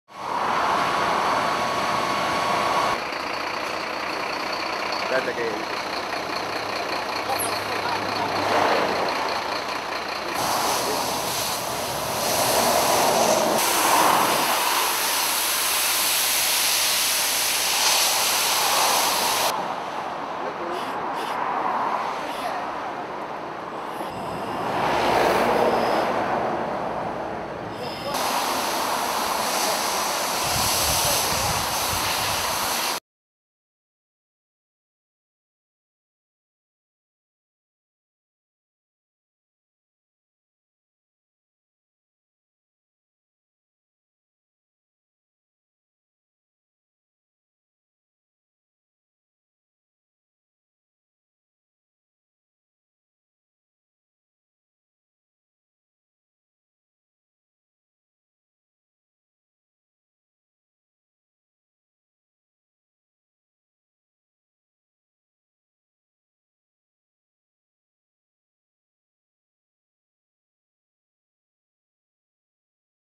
Vigili del Fuoco - Genova A26, 'incendio autoarticolato carico materiale da cancelleria
Questa mattina i Vigili del fuoco hanno operato sull’autostrada A26, in direzione Nord, per l'incendio di un autoarticolato carico di materiale da cancelleria.